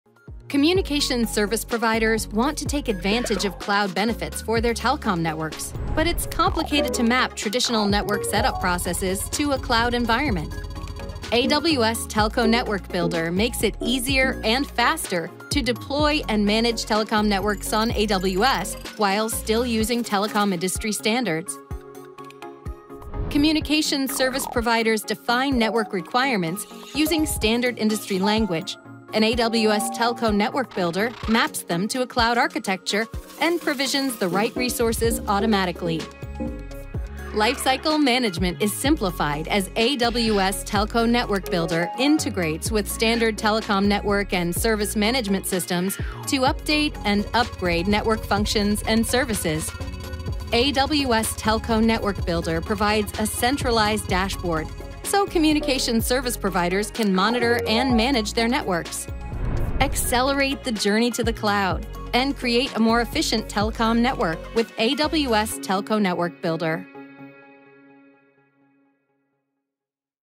Introducing AWS Telco Network Builder | Amazon Web Services
Mapping telco network requirements to a cloud environment is complicated. With AWS Telco Network Builder, Communication Service Providers (CSPs) define network requirements using telecom industry standards, then AWS Telco Network Builder maps the requirements to a cloud architecture and provisions the right AWS resources automatically.